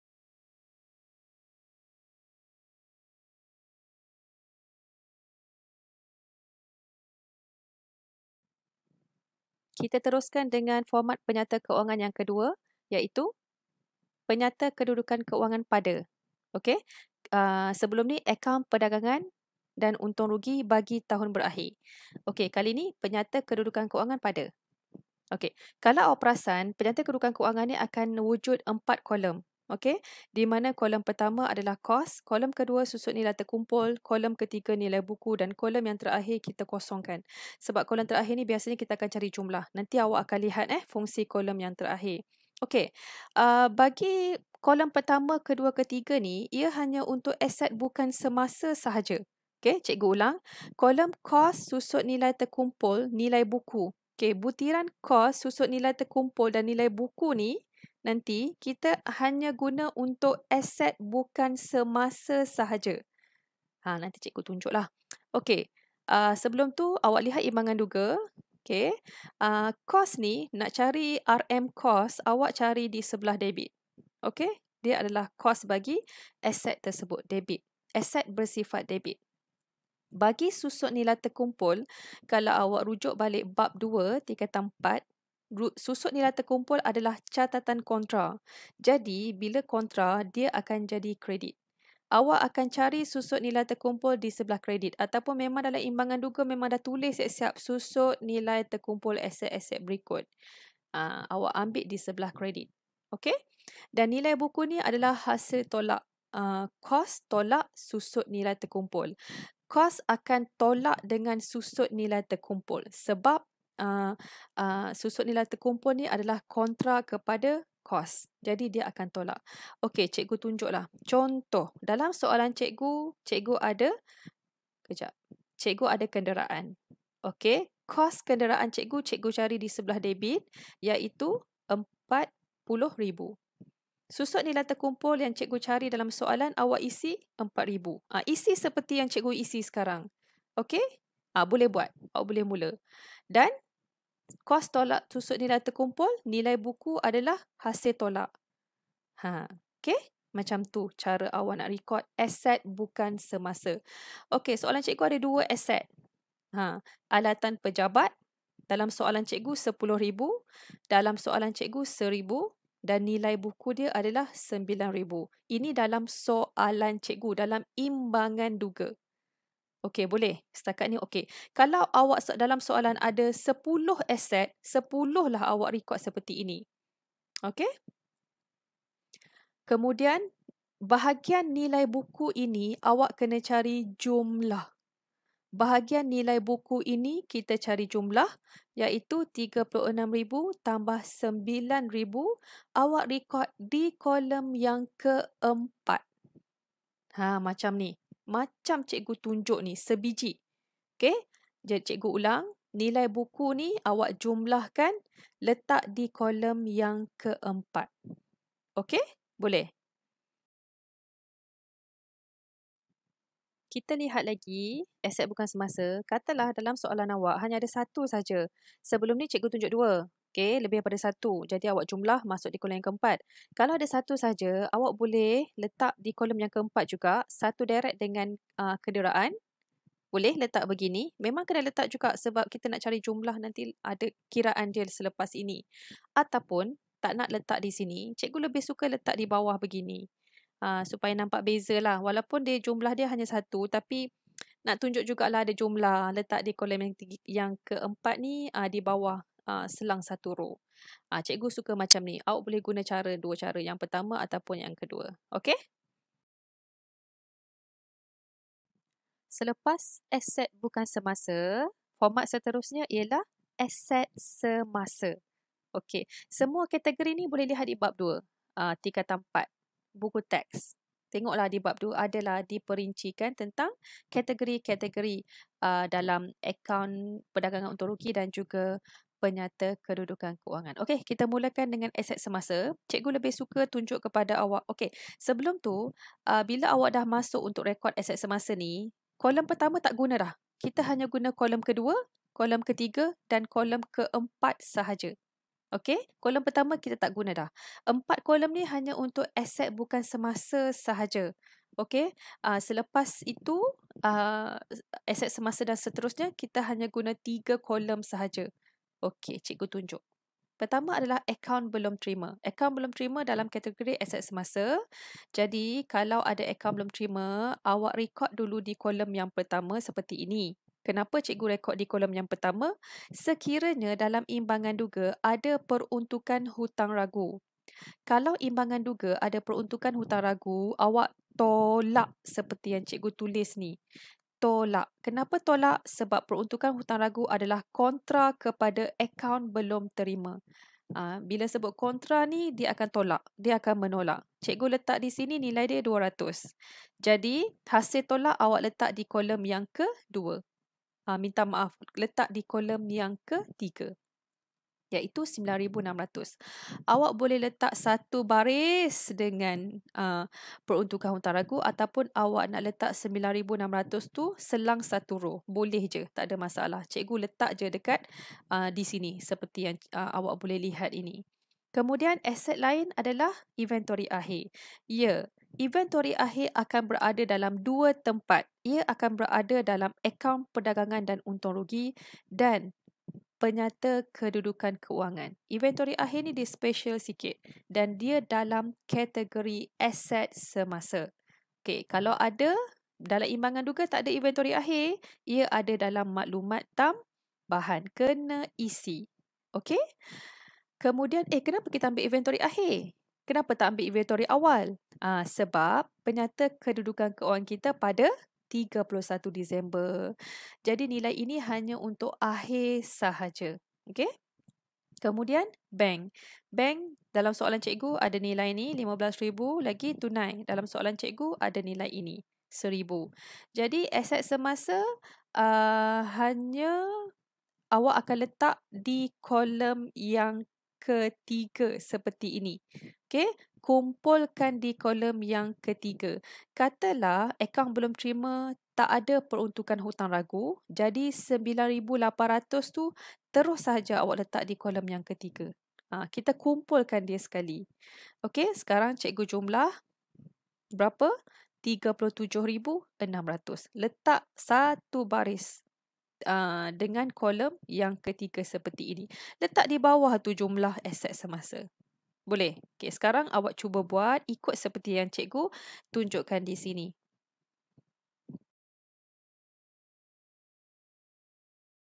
Bil. 06. Jom belajar Penyata Kewangan. Prinsip Perakaunan. Tingkatan 4
Penyediaan Aset bukan semasa dan aset semasa

Dapatkan nota R&R Tingkatan 4 khas cikgu buatkan untuk awak semua.. untuk tempahan boleh click sites cikgu di atas. Akaun tu mudah laaaaaaa